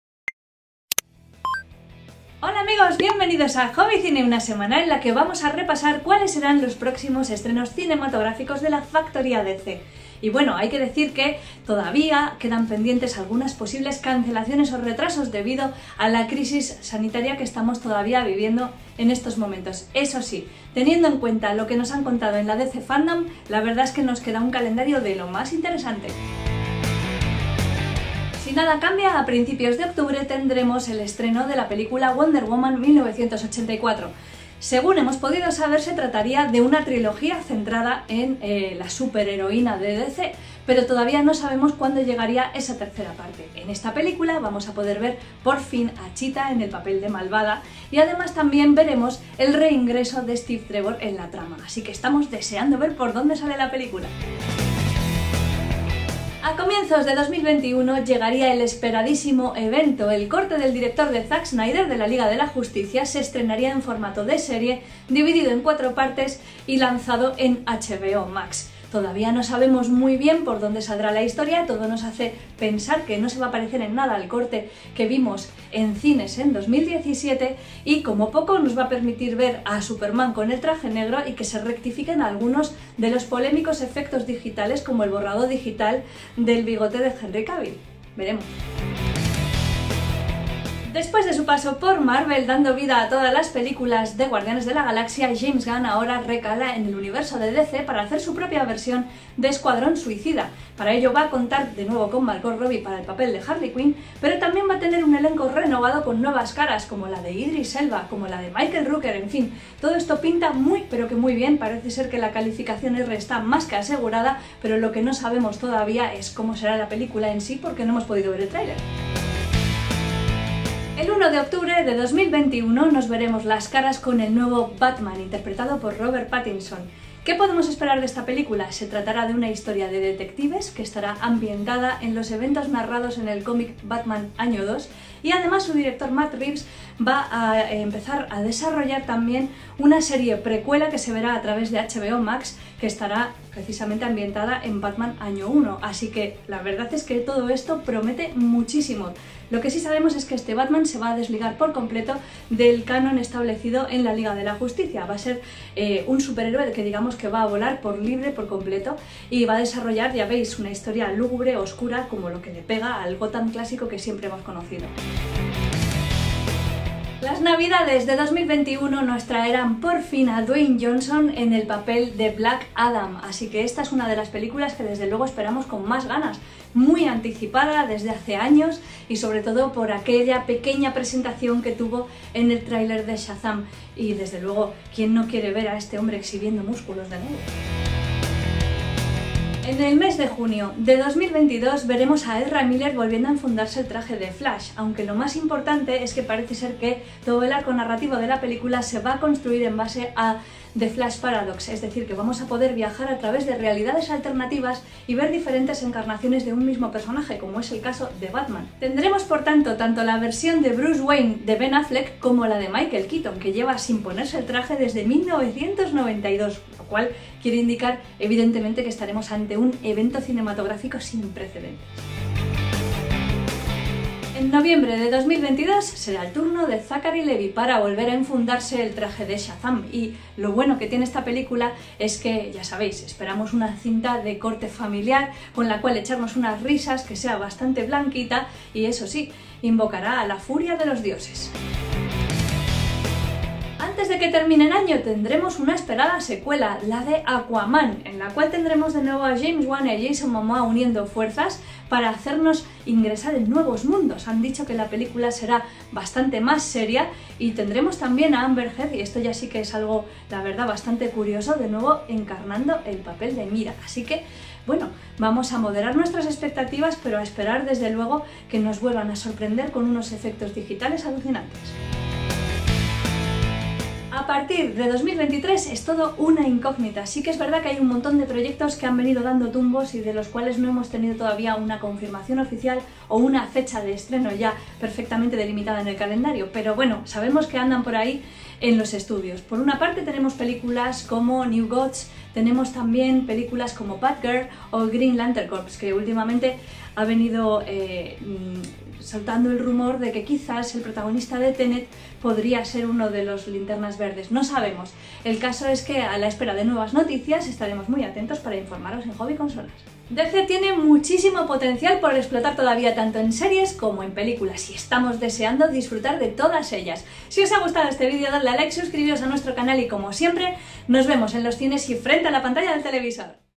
Estrenos confirmados de DC hasta 2023
¡Qué complicado está el mundo del cine! En el transcurso de un par de días, todo puede cambiar. Es el caso de Wonder Woman 1984, que, cuando grabamos este vídeo todavía tenía asignada su fecha de estreno en octubre y que finalmente se ha visto retrasada hasta finales de diciembre... Y con posibilidades de que aún se retrase más, en función de cómo evolucione la crisis sanitaria.
De momento, eso sí, Warner se resiste a pasarla a 2021, así que sigue siendo uno de los blockbusters que se mantienen en este particular 2020 que nos ha tocado vivir. Así, su nueva fecha queda estipulada para el 26 de diciembre mientras que Dune sigue estando programada para el 18 de diciembre.
En fin, en cualquier caso y de mantenerse la situación, será el próximo estreno de DC y a él se sumarán un montón de esperadísimas películas de aquí hasta 2023, entre las que se incluyen las secuelas de Shazam! y Aquaman.